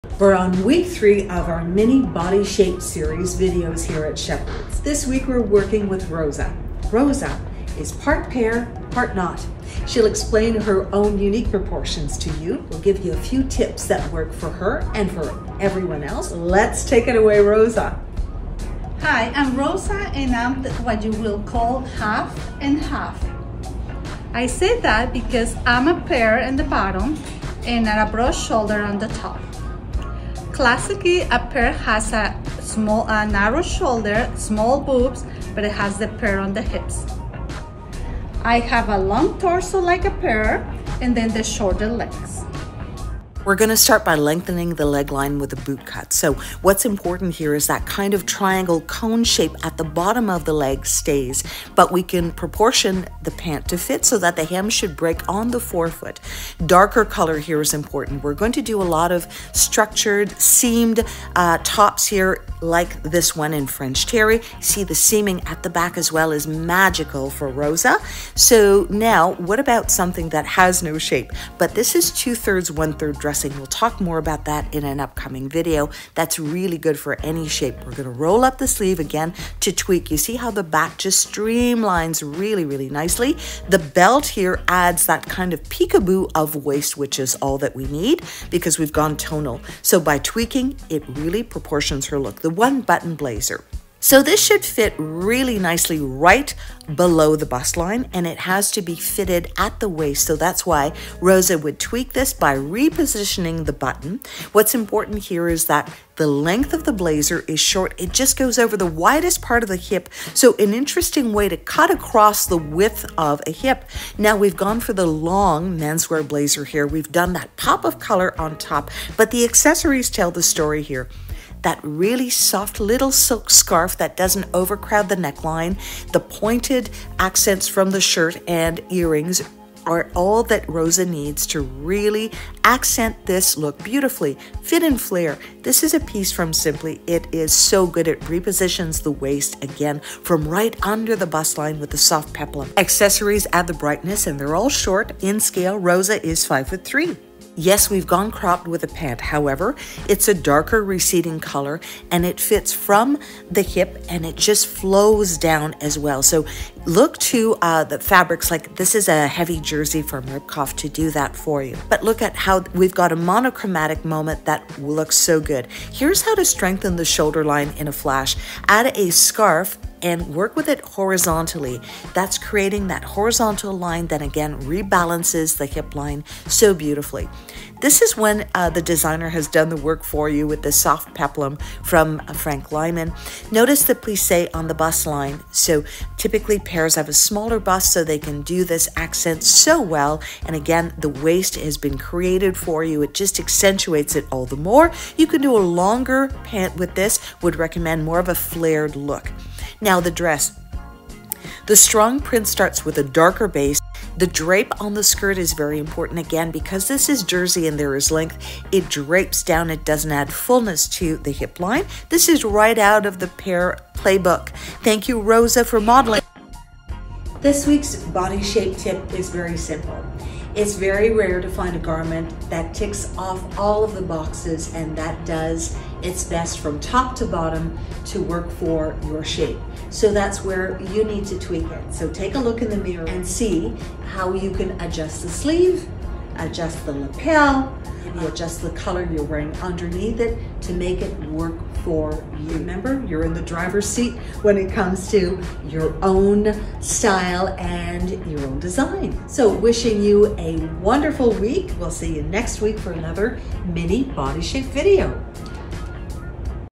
November 7, 2022
It’s all about proportions with Rosa, who’s part Pear Shape Part not.  We’re looking at styling and modern ways to add visual interest or detract with use of colour, shape, texture and design details.